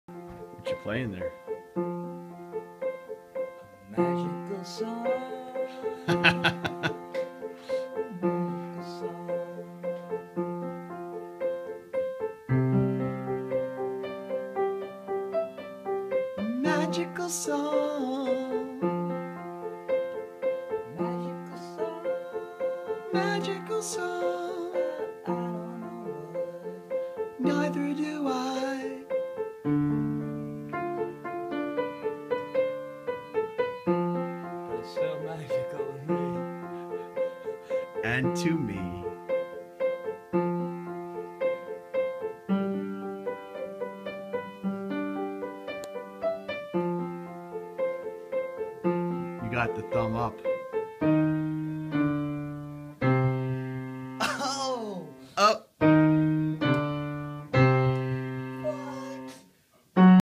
Making Of ... Pete and Jimi Mac...Sing A Magical Song?!?!
Or stream on Spotify RIGHT NOW to listen, Keep Streaming, and KeepRockin

Pete Herger and Jimi Mac work on new record, and sing a new song?!?! Not sure if this one makes the new record. ;-) Let us know what ya think! Might be more appropriate for Elmo to sing on Sesame Street, instead of the Rock n' Rollers! ;-P

For more content, and to follow the Rock n' Roll journey, please join us: